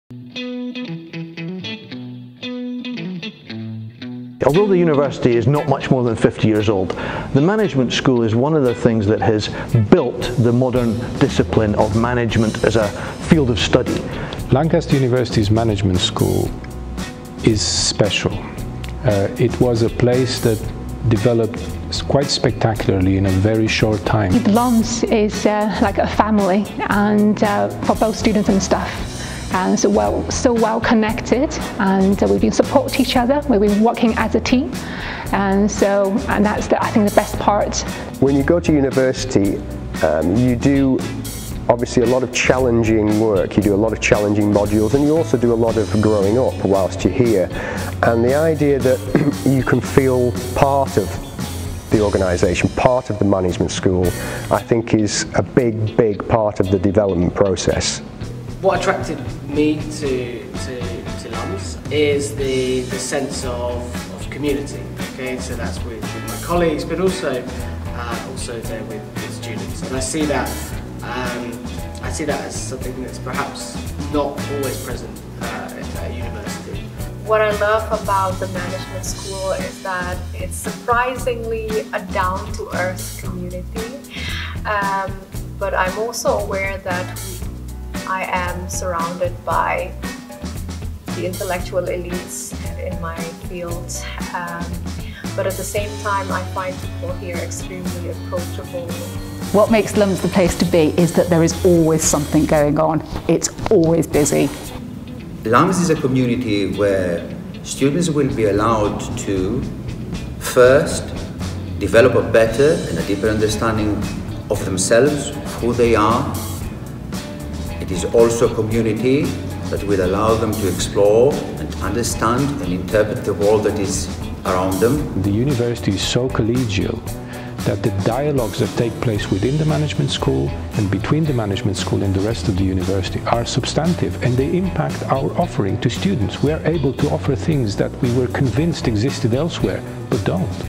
LUMS is the Place to Be
Some of our academic tell us what, for them, makes LUMS the place to be.